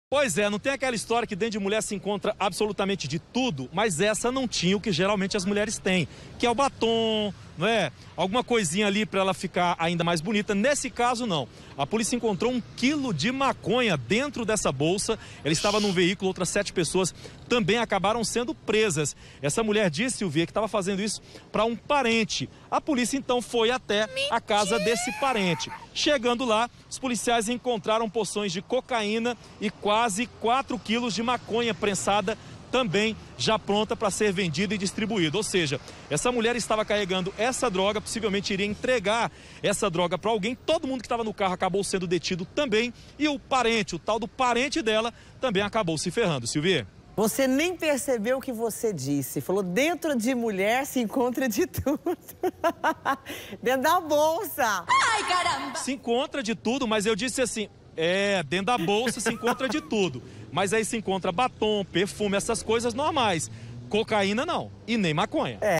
MULHER É PRESA COM DROGA ESCONDIDA NA BOLSA
Policiais do comando de operações de divisas flagraram uma mulher com droga escondida na bolsa.